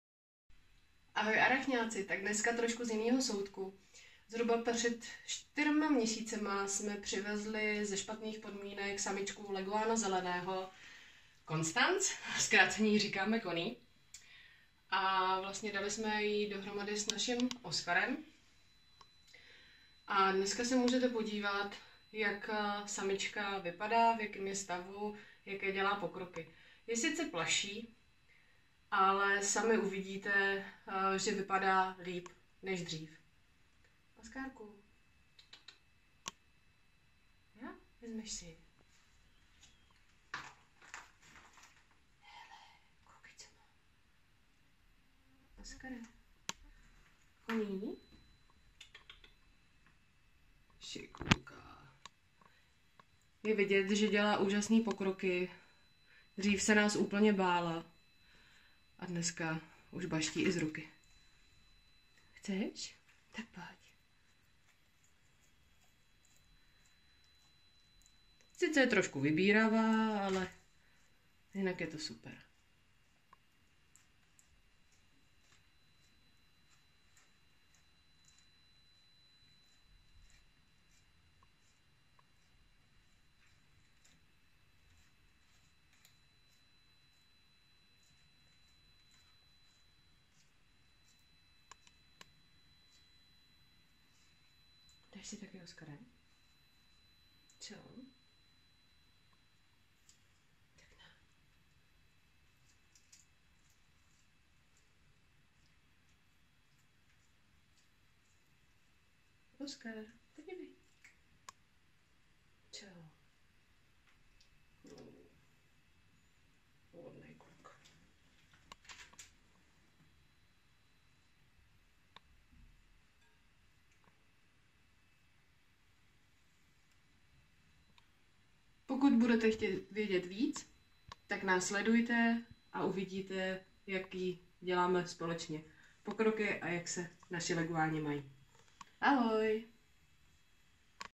Leguánka Connie po 4 měsících u nás - Arachnopark World
Před 4 měsíci se k nám dostala samice ještěra Leguána zeleného. Dostala jmeno Constance a díky tomu, že u predchozich majitelů byla ve zhoršených podmínkách, tak podle toho na začátku vypadalo. Toto terarijní zvíře potrebovalo velkou dávku péče. Na začátku nechtěla ani jíst, takže byl risk si ji k nám vzít, ale my máme rádi výzvy a hlavně rádi pomůžeme zvířeti, které je v nouzi. Samička leguána je už absolutne spokojená a krásně se nám rozdlabala 😍 Tak sledujte naše novinky v Arachnopark World 💪